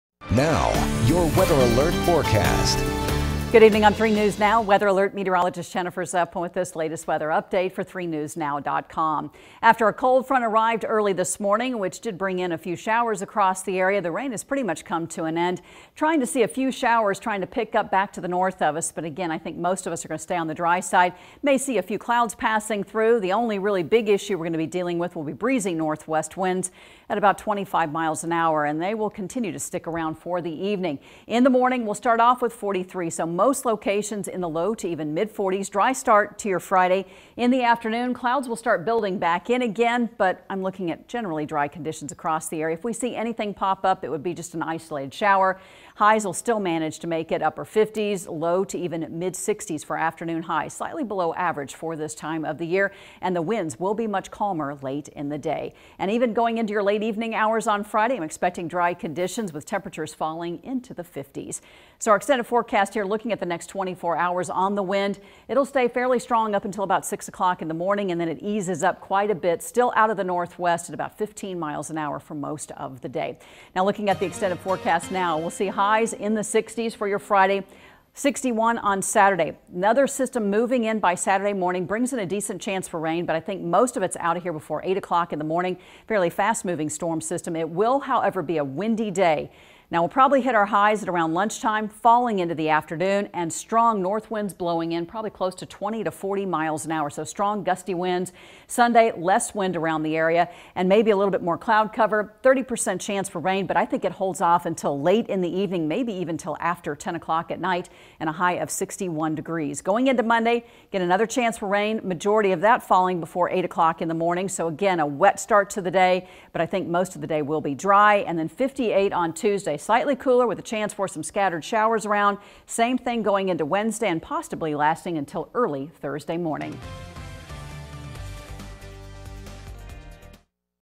Jennifer's Evening Forecast
Breezy tonight with another cold front moving through. Friday morning starts off cool.